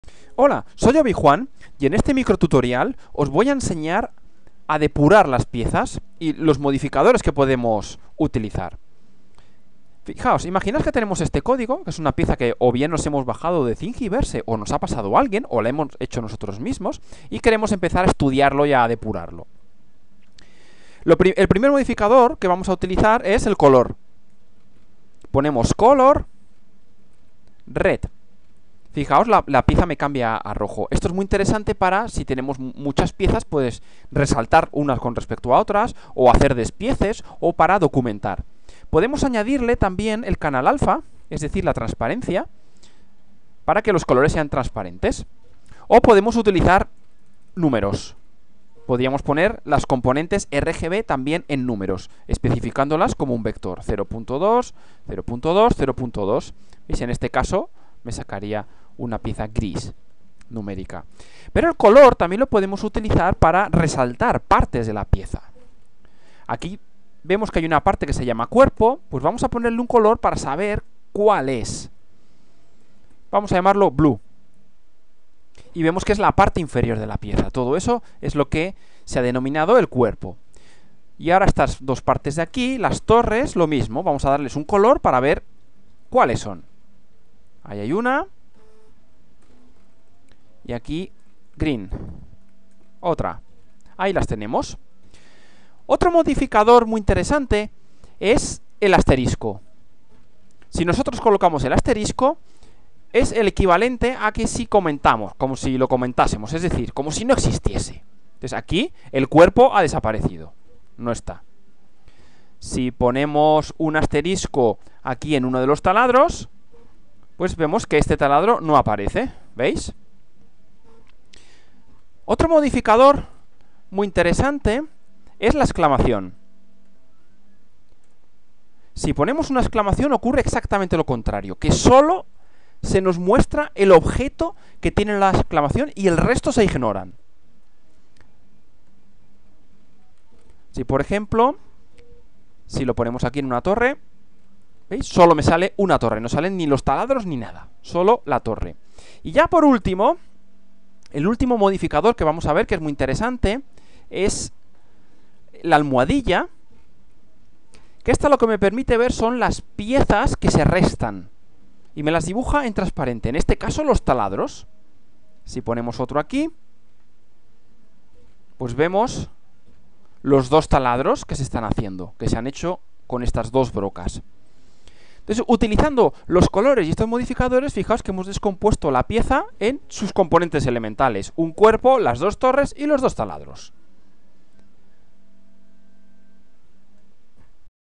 Micro-tutorial OpenScad 13: Depurando (13/14)
Entrega 13 del "micro-tutorial" sobre OpenScad, la herramienta libre y multiplataforma para diseño 3D, basada en scripts. Diseña las piezas para luego imprimirlas en tu impresora 3D open-source